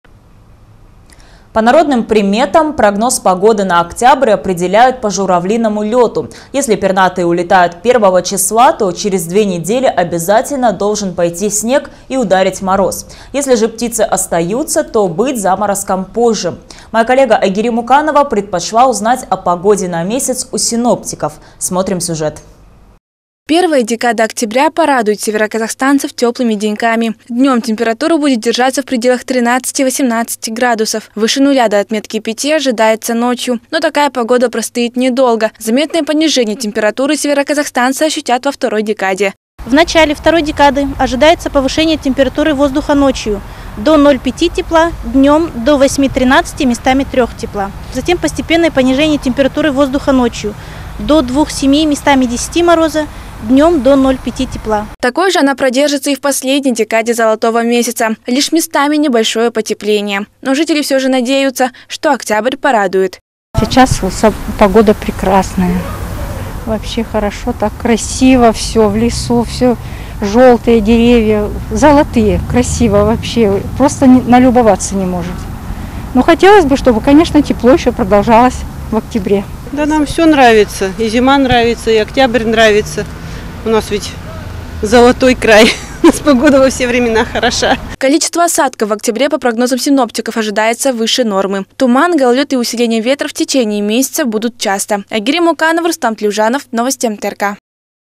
Тепло сохранится лишь в первой декаде октября
По народным приметам прогноз погоды на октябрь определяют по журавлиному лёту. Если пернатые улетают первого числа, то через две недели обязательно должен пойти снег и "ударить" мороз. Если же птицы остаются, то быть заморозкам позже. Моя коллега Айгерим Муканова предпочла узнать о погоде на месяц у синоптиков. Смотрим сюжет.